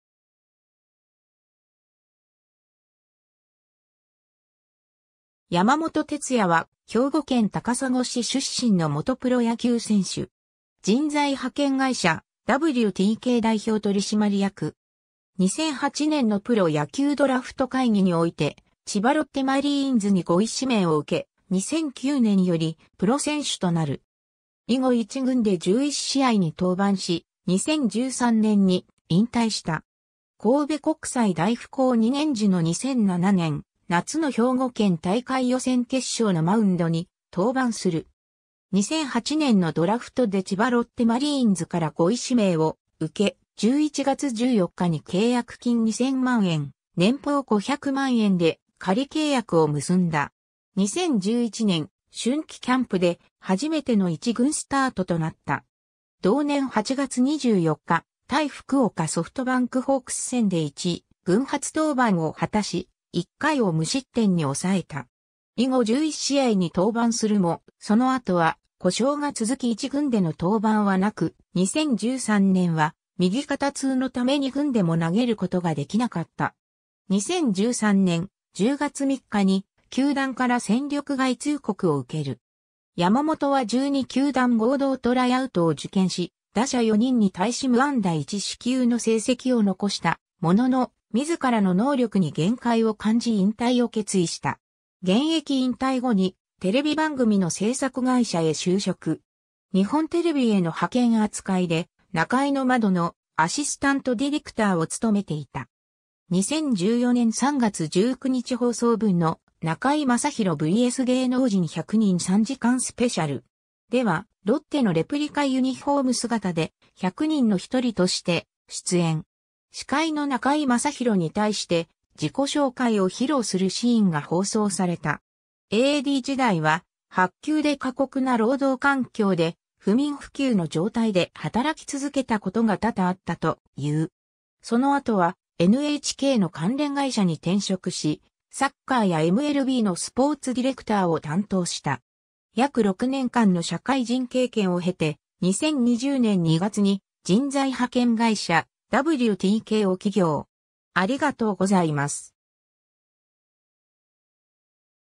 山本徹矢
山本 徹矢（やまもと てつや、1990年6月6日 - ）は、兵庫県高砂市出身の元プロ野球選手（投手）。
人材派遣会社・W-TK代表取締役。
 2008年のプロ野球ドラフト会議において千葉ロッテマリーンズに5位指名を受け、2009年よりプロ選手となる。
以後1軍で11試合に登板し、2013年に引退した。
 神戸国際大附高2年時の2007年、夏の兵庫県大会予選決勝のマウンドに登板する。
 2008年のドラフトで千葉ロッテマリーンズから5位指名を受け、11月14日に契約金2000万円、年俸500万円で仮契約を結んだ。
 2011年、春季キャンプで初めての一軍スタートとなった。
同年8月24日、対福岡ソフトバンクホークス戦で一軍初登板を果たし、1回を無失点に抑えた。
以後11試合に登板するも、その後は故障が続き1軍での登板は無く、2013年は右肩痛のため2軍でも投げることができなかった。
 2013年、10月3日に球団から戦力外通告を受ける。
山本は12球団合同トライアウトを受験し、打者4人に対し無安打1四球の成績を残したものの、自らの能力に限界を感じ引退を決意した。
 現役引退後に、テレビ番組の制作会社へ就職。
日本テレビへの派遣扱いで、『ナカイの窓』のアシスタントディレクターを務めていた。
2014年3月19日放送分の『中居正広VS芸能人100人　3時間スペシャル!』では、ロッテのレプリカユニフォーム姿で、「100人の1人」として出演。
司会の中居正広に対して、自己紹介を披露するシーンが放送された。
AD時代は薄給で過酷な労働環境で、不眠不休の状態で働き続けたことが多々あったという。
 その後はNHKの関連会社に転職し、サッカーやMLBのスポーツディレクターを担当した。
 約6年間の社会人経験を経て、2020年2月に人材派遣会社「W-TK」を起業。